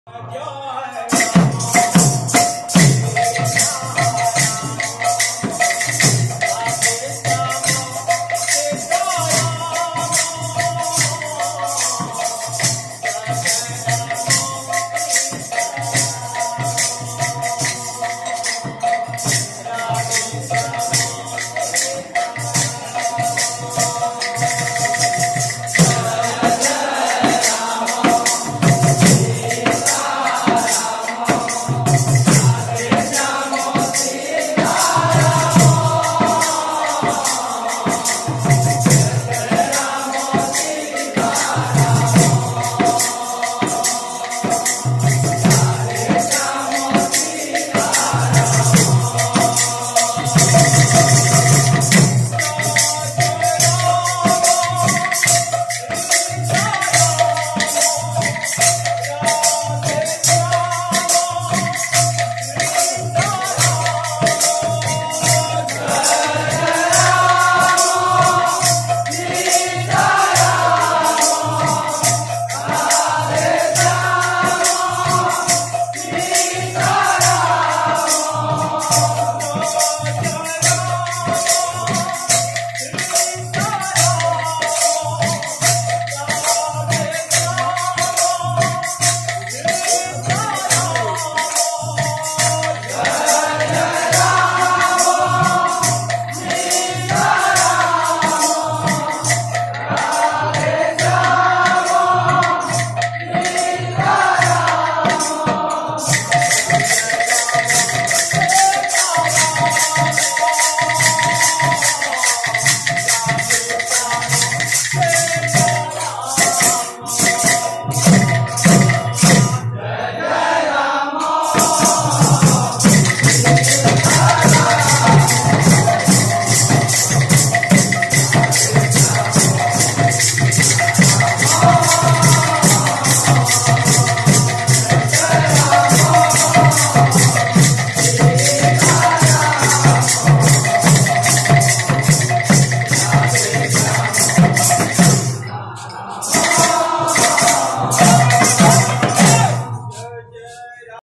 Devalsurra mandalii (Trada kirtan)
Love for jann

Ajj devalsurra me ram saptah ka 5th day .

Trada kirtan mandli .